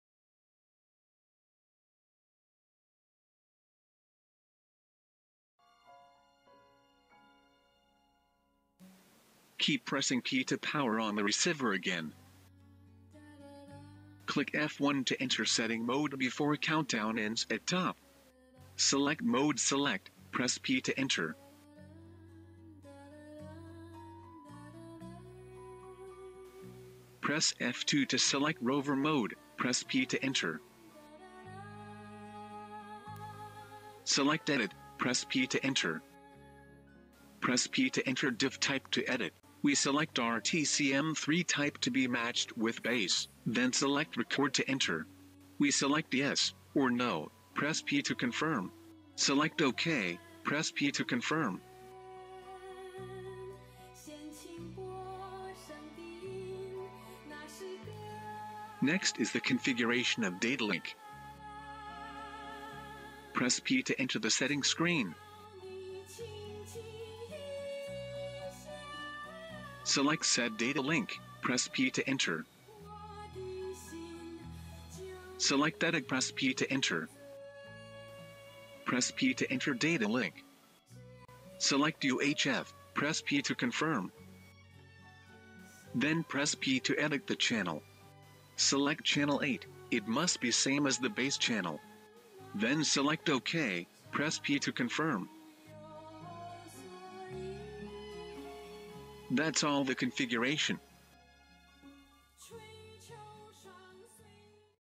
S86T Rover Internal
Configuracion de GNSS South  S86T en modo Rover con radio interno